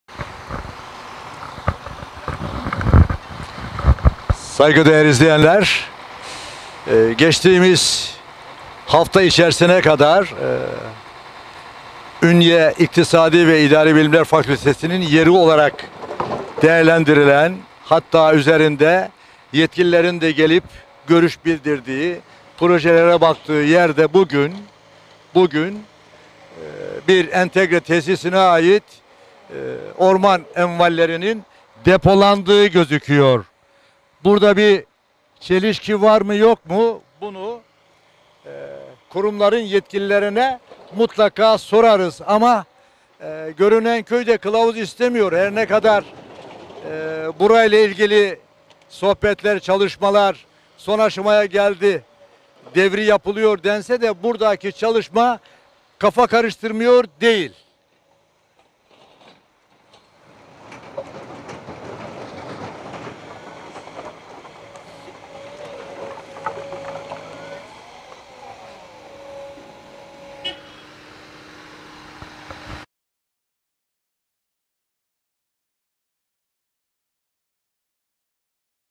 ÜNİVERSİTE YERLEŞKESİNDE NELER OLUYOR ?
Fakülte yeri olarak Ünyenin ve siyasilerimizin Ankarayı su yaptığı bu özel ve önemli konu Ünyeliler MODA TV İhbar Hattına  ihbarda bulunarak,ihbarı alıp olay yerine intikal eden Moda Tv ekibi söz konusu olayla ilgili görüntüleri alıp gerekli çalışmayı yaptıktan sonra telefon ile iritibat kurduğu Ünye belediye Başkanı Hüseyin Tavlı ile fikir alış verişinde bulundu.
Çok sayıda İnsanımızın uzun yıllar ve halen daha çalışarak ekmek yediği  bölgemizideki önemli sanayi kuruluşlarımızdan biri olan Kastamonu Entegre Tesisine 3 aylık süre sonunda temelini atacağımız Fakültemizin yollarının asfalt yapılması  kaydı şartıyla kullanılmak üzere tahsis edildiğini söyleyen Ünye belediye Başkanı Hüseyin Tavlı” Ünyeli hemşehrilerimiz müsterih olsunlar. Söz verdiğimiz her konunun AK Parti Genel Başkan Yardımcısı Hemşehrimiz Prof.Dr.Numan Kurtulmuş bey başta olmak üzere Ünyeli hemşehrimiz Dışişleri bakan yardımcımız Yavuz Selim Kıran beyin destekleriyle ile sonuna kadar takipçisiyiz.Yüce Allah’ın izni keremiyle  alt yapı çalışmaları tamamlanan Ünye Fakültemizin temeli Nisan 2020’de  sürpriz bir şekilde Ünyeli İş Adamlarımızca atılacak “dedi.